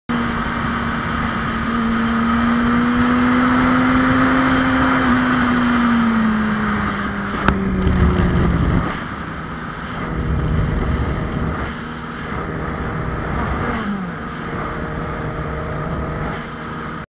R.I.P σπουργτάκι
ατυχημα με ενα σπουργιτακι και η αντιδραση της φιλης μου παρολο που εκανα προσπαθεια να το αποφυγω